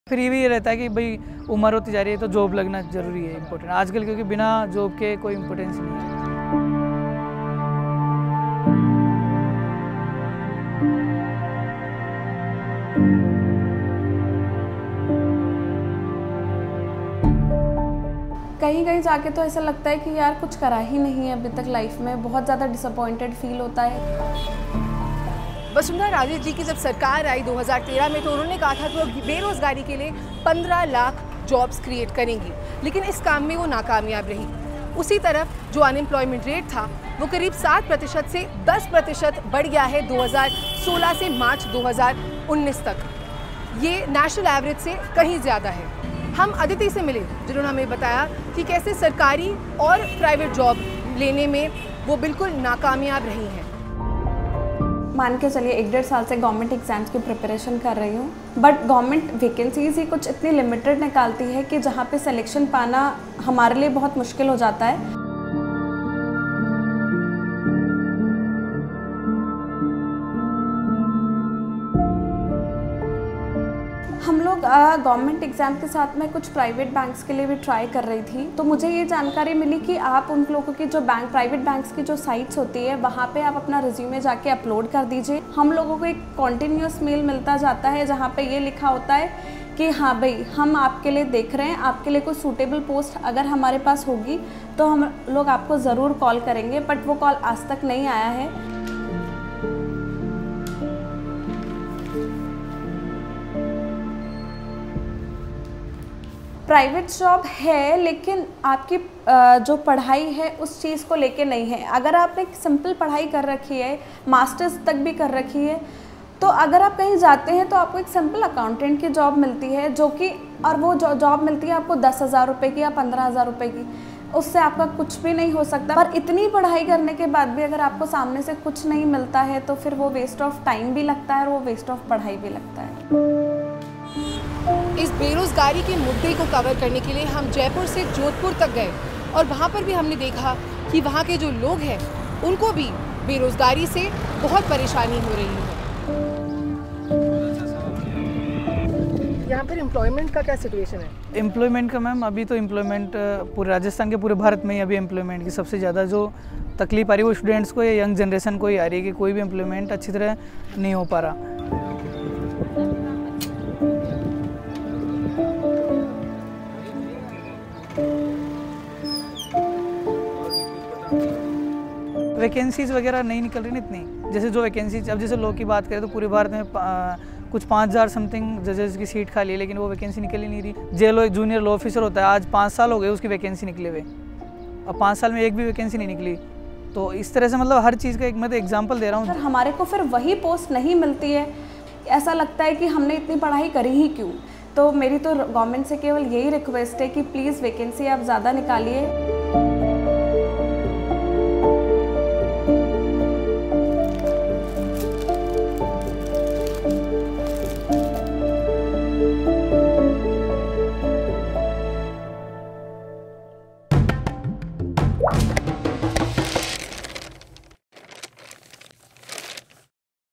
Election 2019- Rajasthan में बेरोजगारी के मुद्दे पर BJP को कितना नुक्सान? | Quint Hindi
जयपुर में 28 साल कि अदिति के पास बेचलर ऑफ़ कॉमर्स, मास्टर इन कॉमर्स और बेचलर इन लॉ की डिग्रियां हैं लेकिन अब भी वो नौकरी ढूंड रही हैं क्विंट से बातचीत में उन्होंने कहा- 'कभी-कभी लगता है कि मैंने जिंदगी में कुछ नहीं किया, हम बहुत निराश हैं'